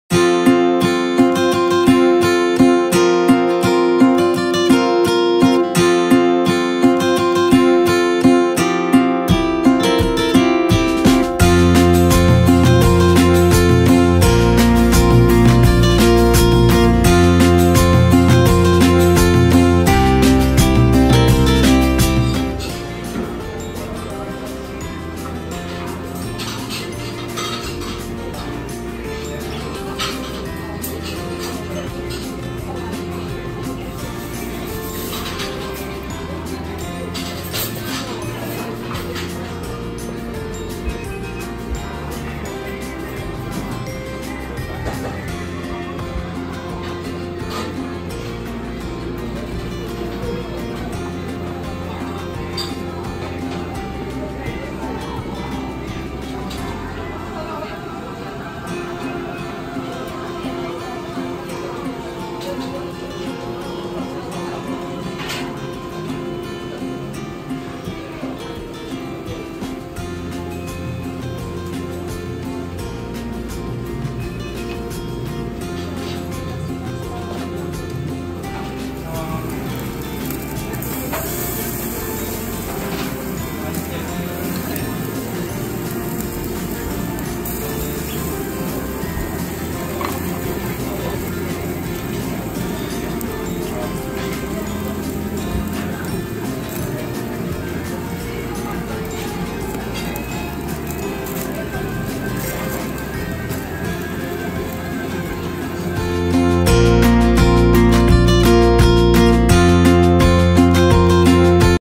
焼肉ライク（新宿西口店）
1人焼肉専門店に行ってきました！！これは専門店だけあって1人で入りやすいし、焼肉をゆっくり堪能できますね！素敵やん！！